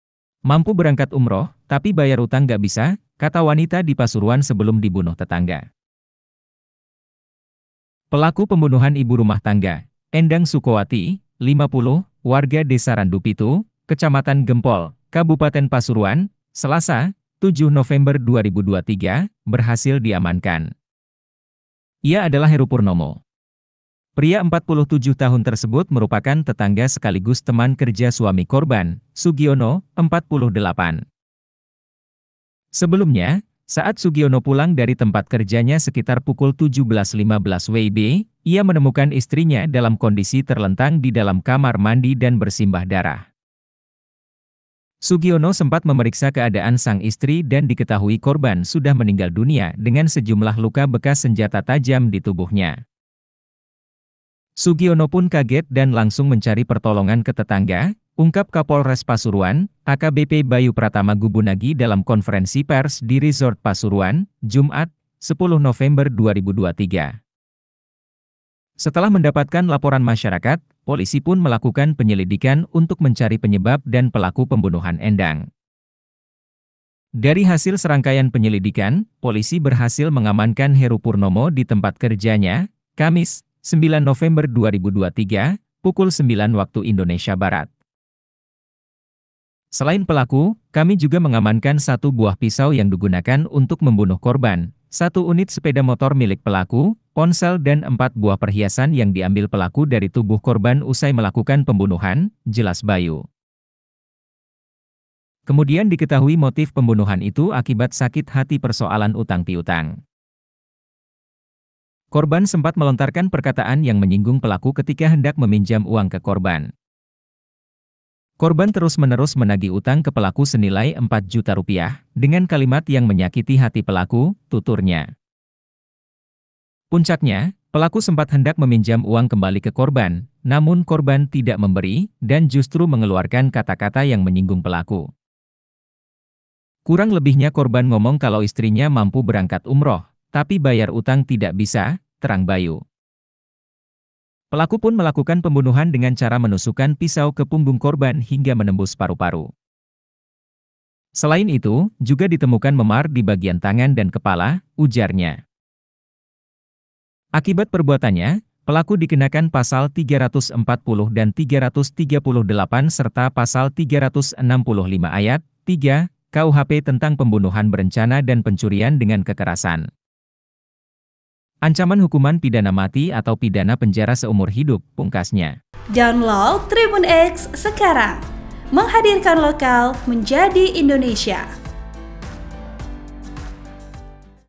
'Mampu Berangkat Umroh, Tapi Bayar Utang Gak Bisa' Kata Wanita di Pasuruan Sebelum Dihabisi Tetangga
Pelaku pembunuhan ibu rumah tangga, Endang Sukowati (50) warga Desa Randupitu, Kecamatan Gempol, Kabupaten Pasuruan, Selasa (07/11/2023), berhasil diamankan.